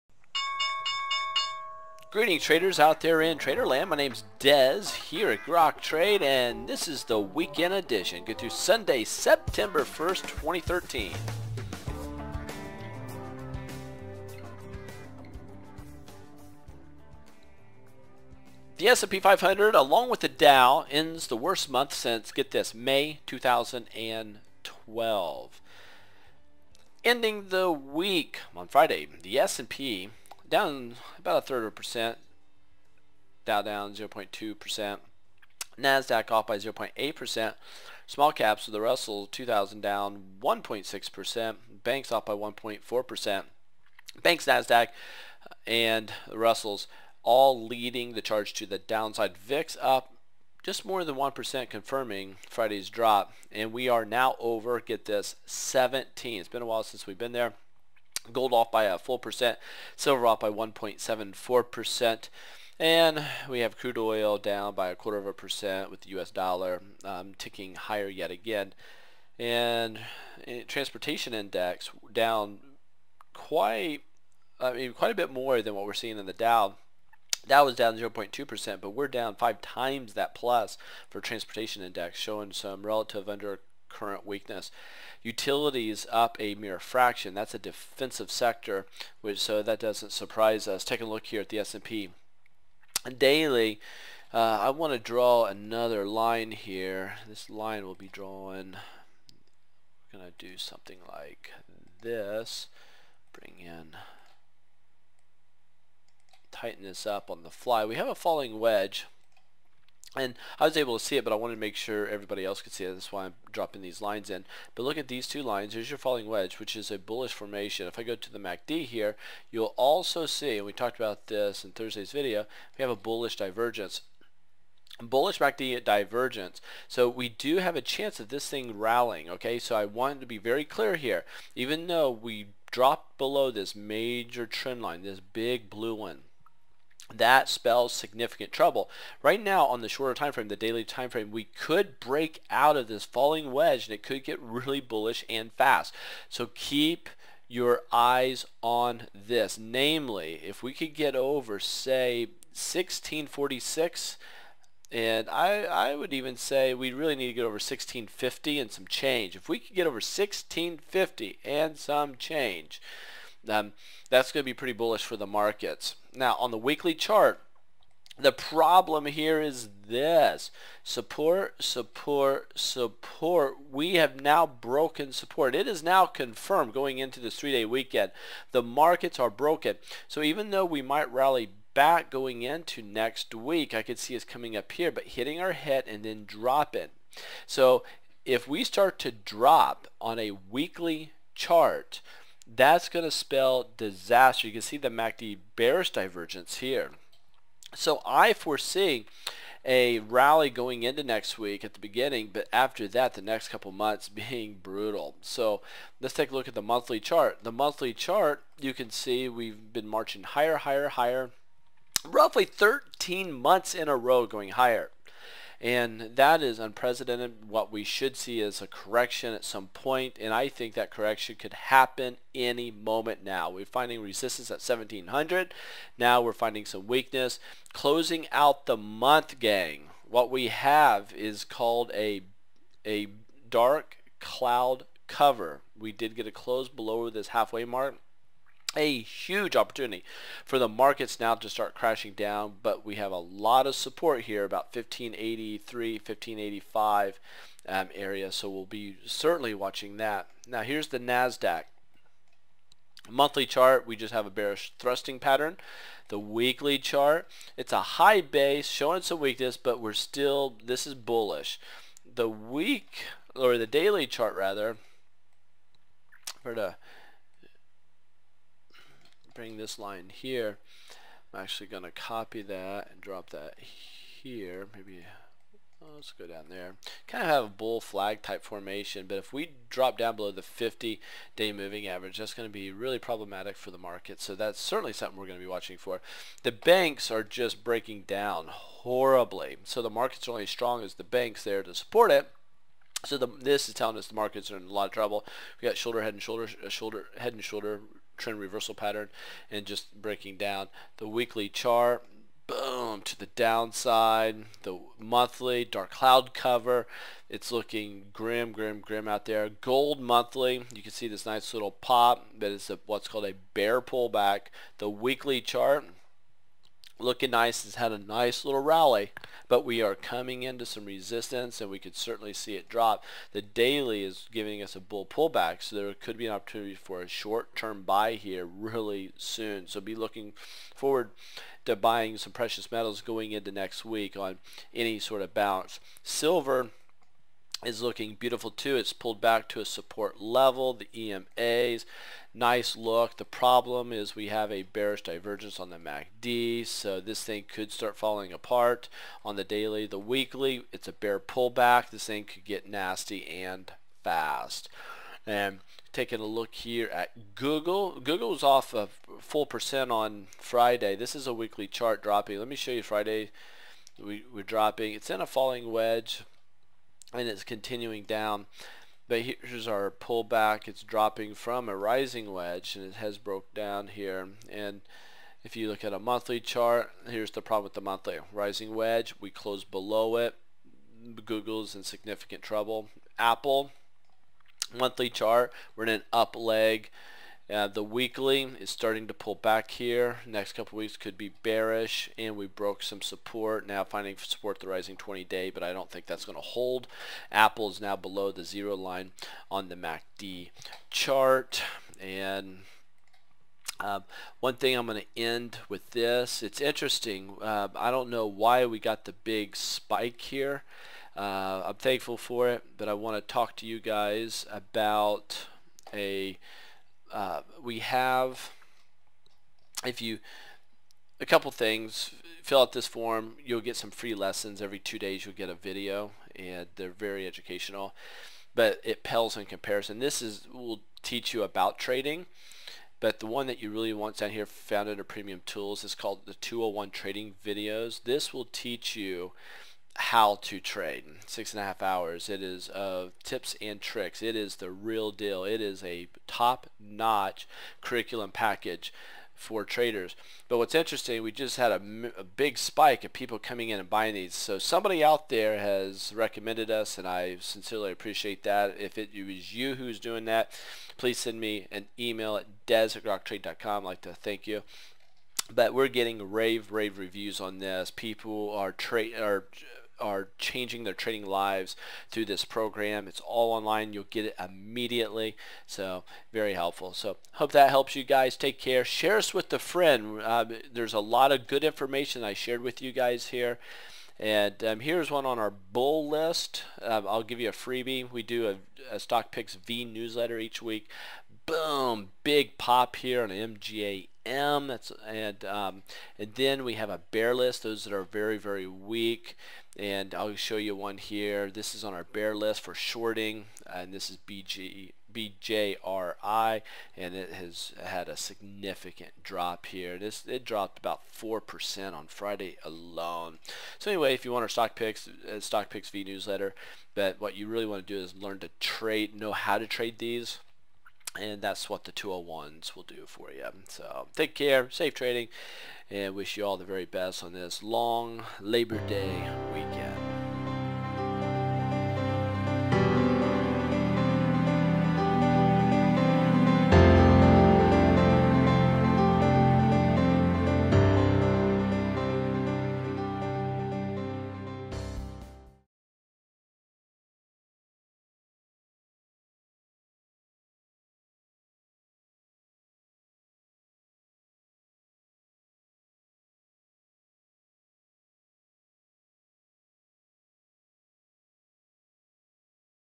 9.1.13 Weekend Edition
IMPORTANT:  Both the S&P and Dow recording their steepest monthly drop since May 2012 before a long weekend. Weekly charts breaking down; monthly charts might be topping out. Precious metals ready for short-term bounce. VIX at 17, which is higher then recent norms.